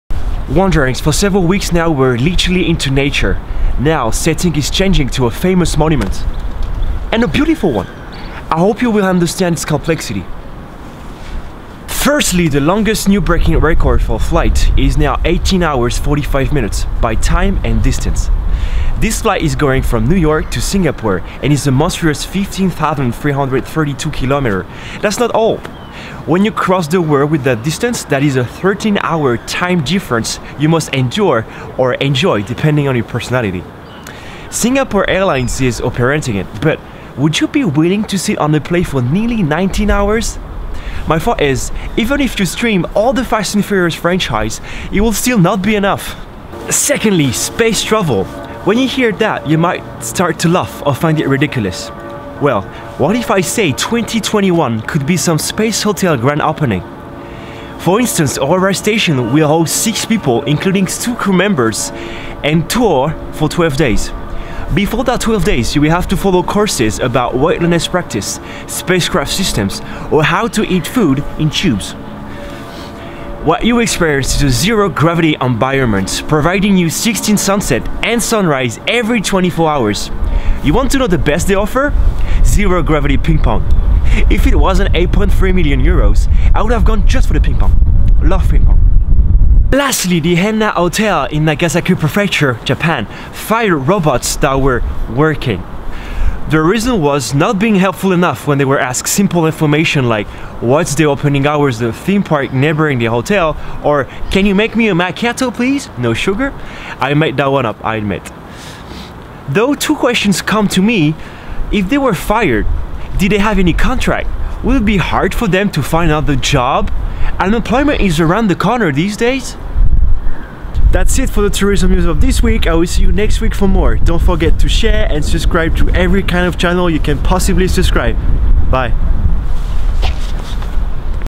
Weekly News #11 - Long Flight, Space Hotels and Robots Fired
Semaine du 4 février 2019.

4th of February week 2019.

The longest flight record, some space hotels might open in 2021 and robots have been fired in an hotel, Japan.